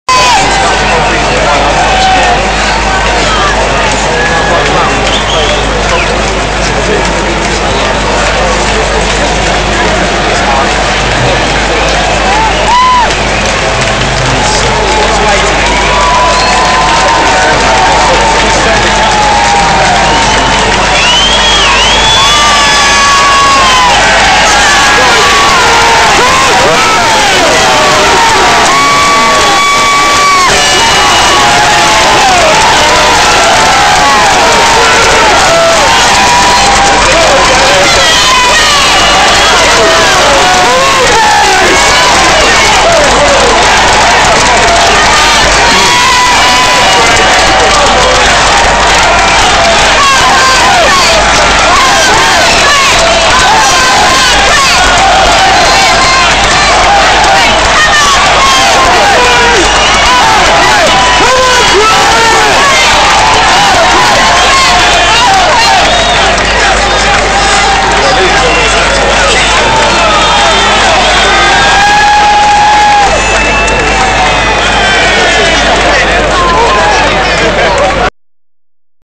Aviva Premiership Final 2012
Harlequins players arriving at Twickenham Stadium!!